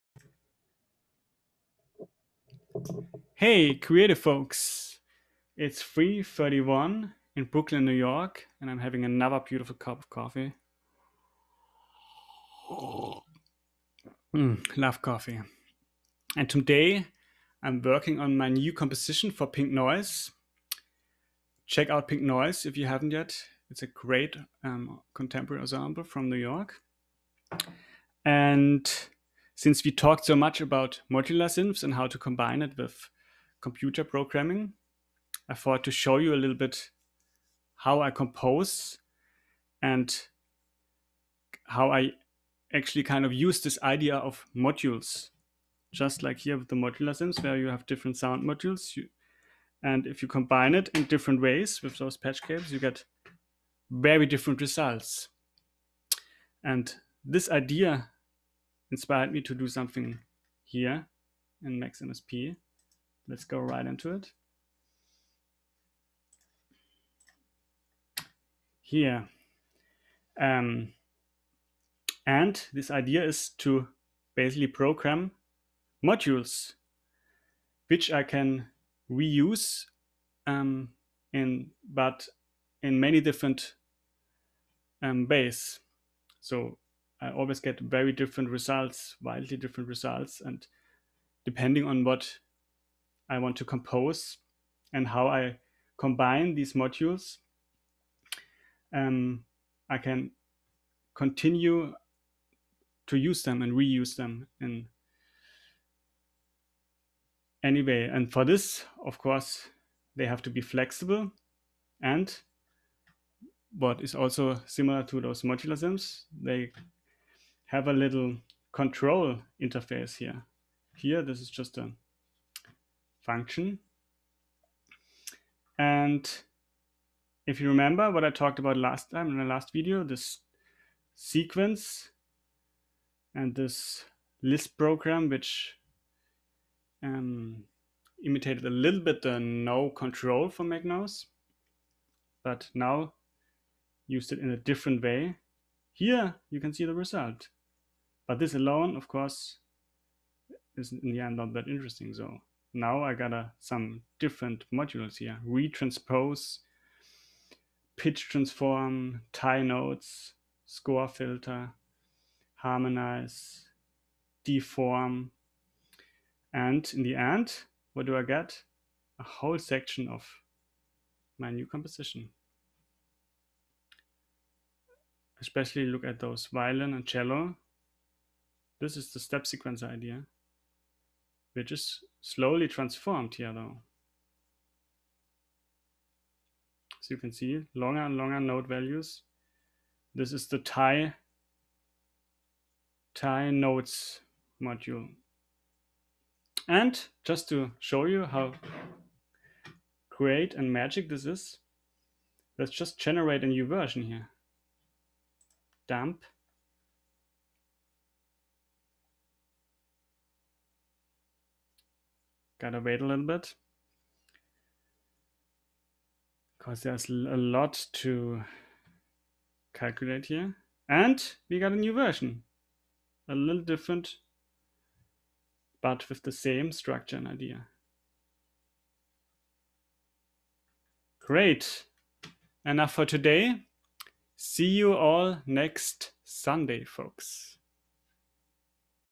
Simon Kanzler's Creative Coffee Time (modular composition in MaxMSP 01/30/2022
for everyone that loves coffee, art, experimental and adventurous music, electronics, modular synths, audio programming and algorithmic composition in MaxMSP